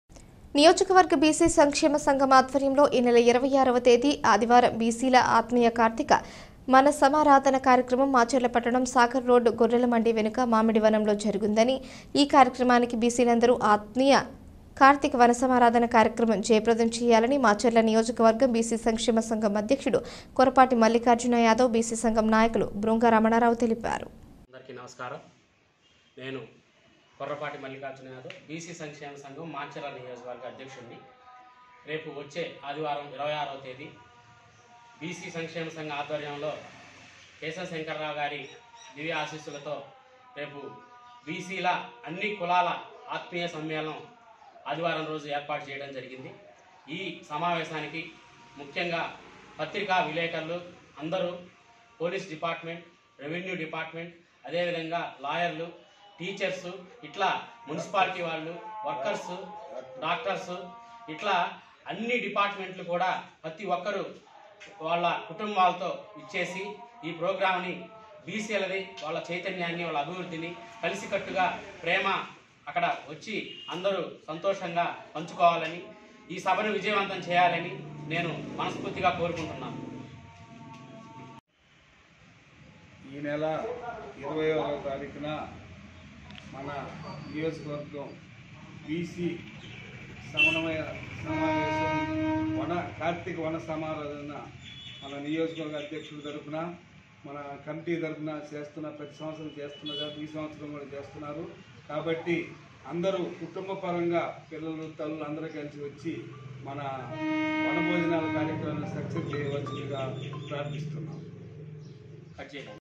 మాచర్ల నియోజకవర్గ బీసీ సంక్షేమ సంఘం ఆధ్వర్యంలో ఈనెల 26వ బీసీల ఆత్మీయ కార్తీక మన సమారాధన కార్యక్రమం
నియోజకవర్గ బీసీ సంక్షేమ సంఘం ఆధ్వర్యంలో ఈనెల 26వ తేదీ ఆదివారం బీసీల ఆత్మీయ కార్తీక మన సమారాధన కార్యక్రమం మాచర్ల పట్టణం సాగర్ రోడ్డు గొర్రెల మండి వెనక మామిడి వనంలో జరుగునని ఈ కార్యక్రమానికి బీసీ లందరూ బీసీల ఆత్మీయ కార్తీక వన సమారాధన కార్యక్రమం జయప్రదం చేయాలని మాచర్ల నియోజకవర్గం బీసీ సంక్షేమ సంఘం అధ్యక్షుడు కొరపాటి మల్లికార్జున యాదవ్, బీసీ సంఘం నాయకులు బృంగరమణారావు తెలిపారు